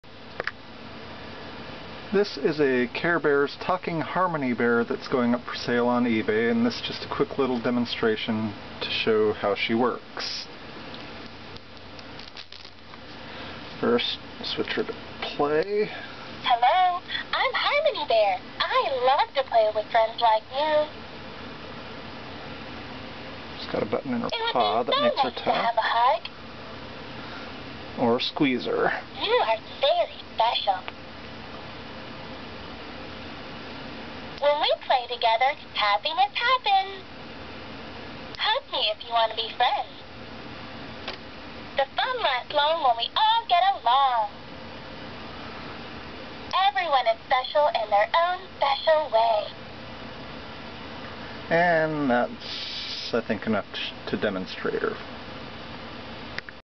eBay Item Demo - Care Bears Talking Plush Harmony Bear
Demonstration video for a talking plush Care Bears Harmony Bear that is going up for sale.